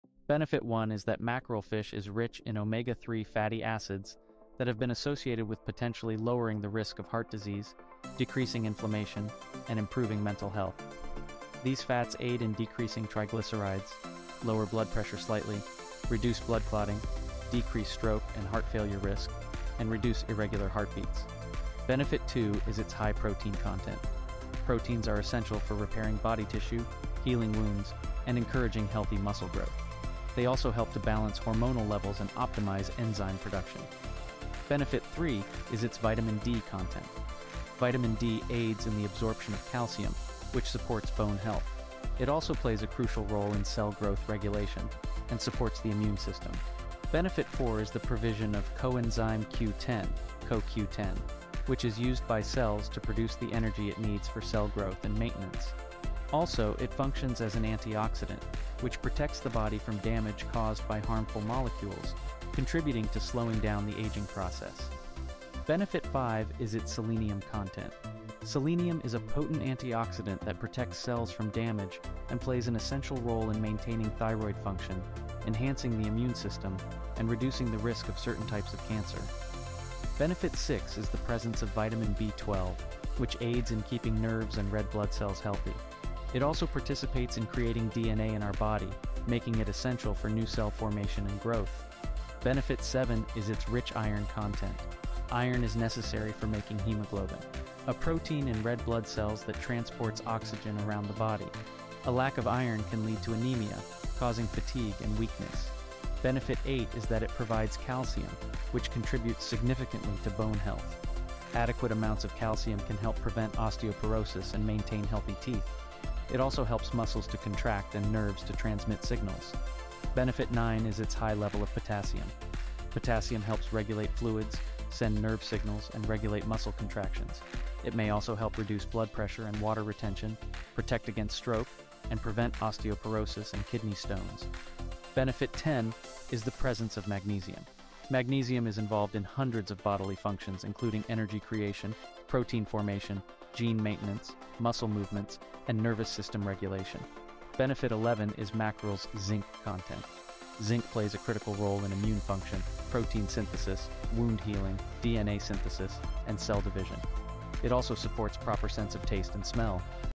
11 Wonders of Mackerel Fish
Uncover the incredible health benefits of mackerel in this video! Learn how its rich omega-3 fatty acid content can potentially lower the risk of heart disease, improve mental health, and decrease inflammation. Discover its high protein and vitamin D contents which are essential for tissue repair, bone health, and immune support. Explore how Coenzyme Q10, selenium, and vitamin B12 in mackerel contribute to energy production, thyroid function, and red blood cell health. Understand how its iron, calcium, potassium, magnesium, and zinc contents promote optimal body functioning from bone health to nervous system regulation. Dive into the nutritious world of mackerel and transform your diet today!